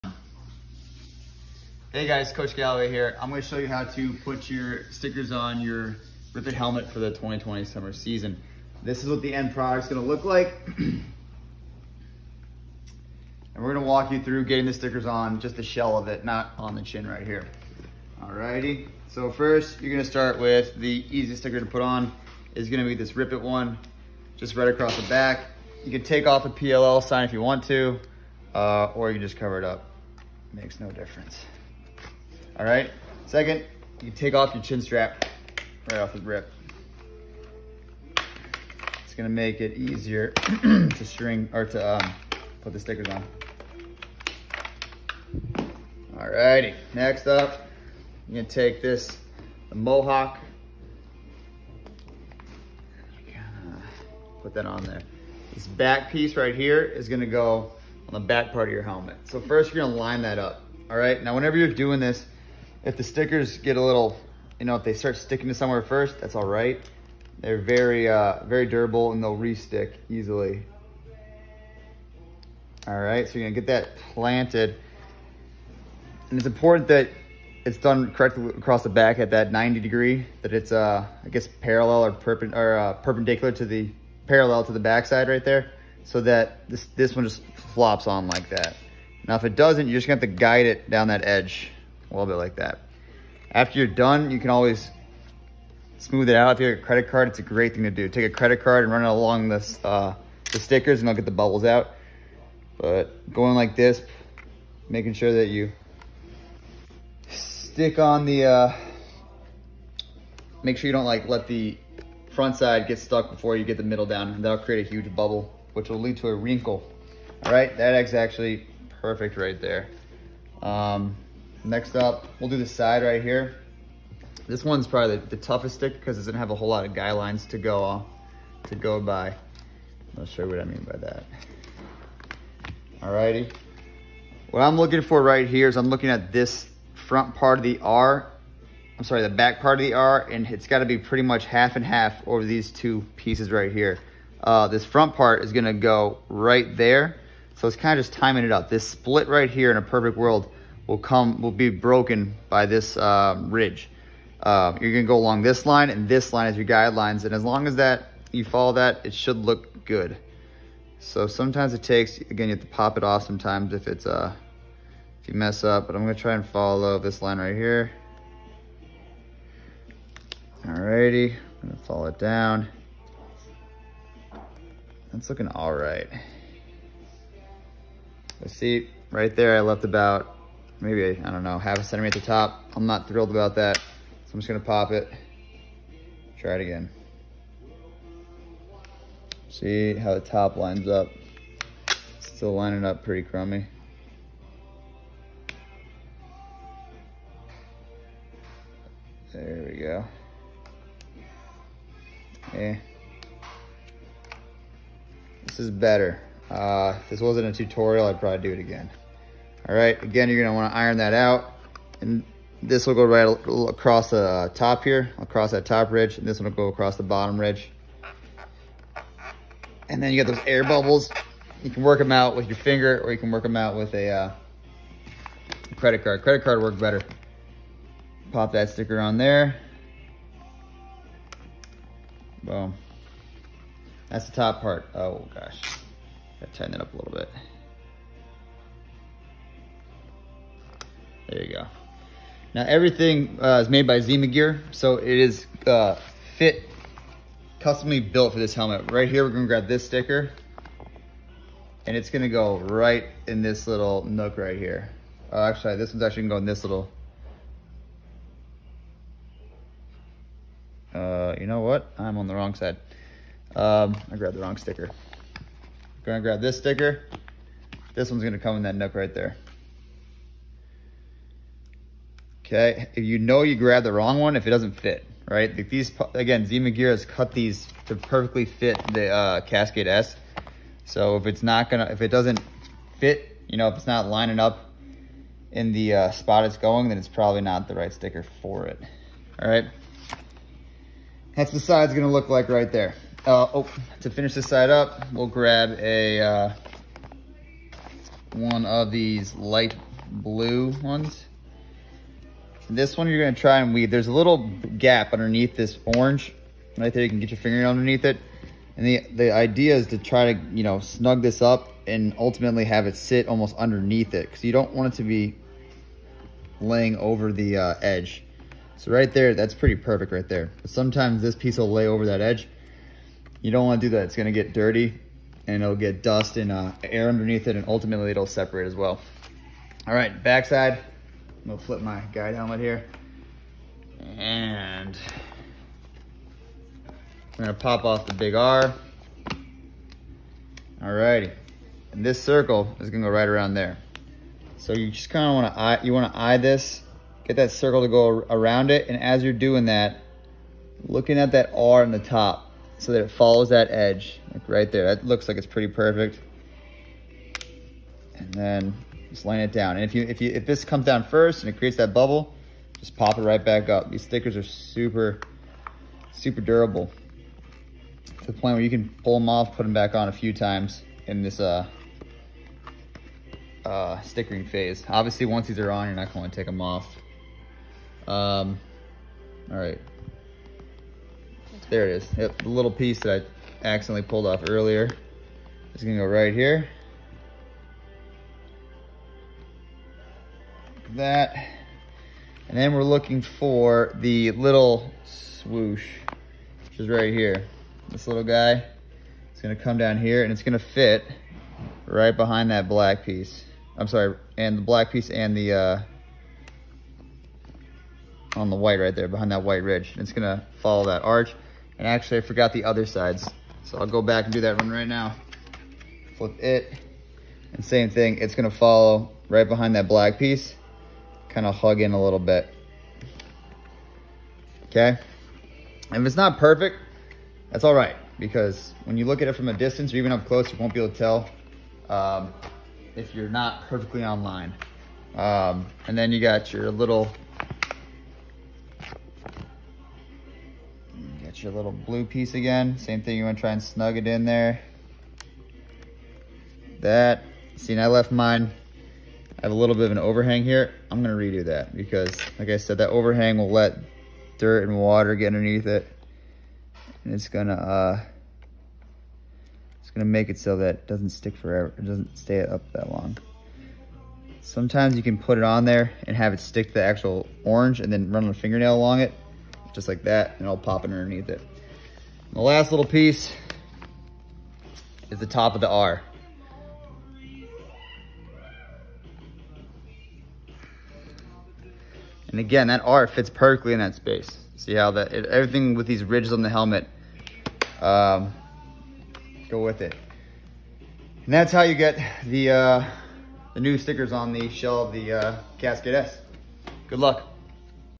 Rip It Helmet Sticker Video
How to properly place stickers on Cascade S helmet 2020